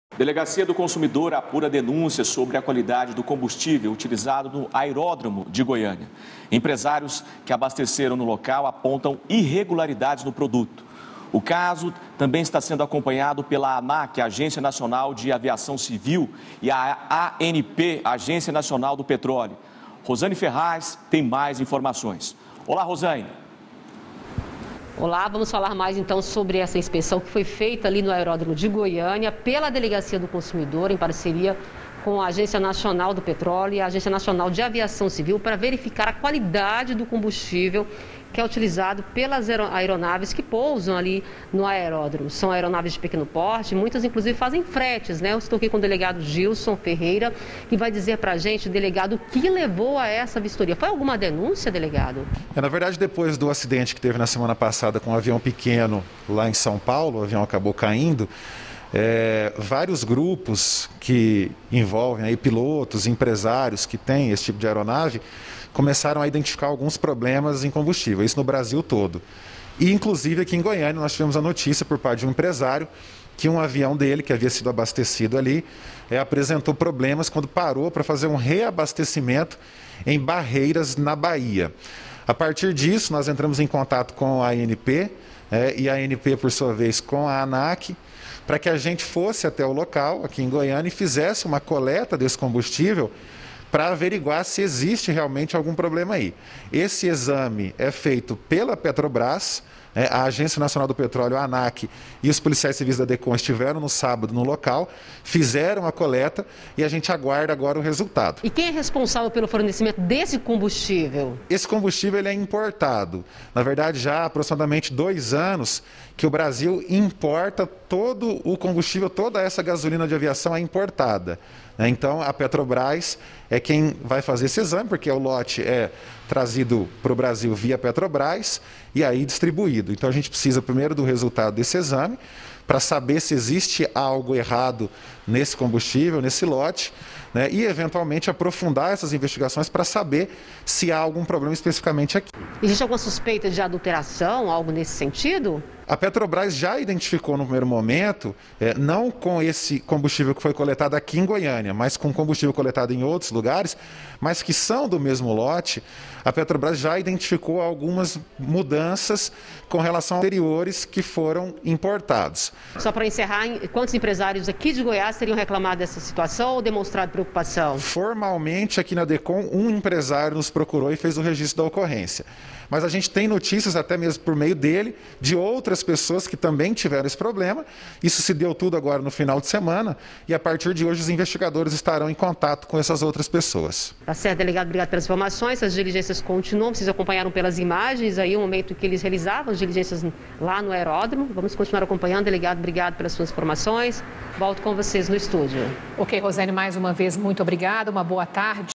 JMD (13/07/20) Qualidade do combustível do aeródromo de Goiânia é testada
Delegacia do consumidor apurou denúncias sobre a qualidade do combustível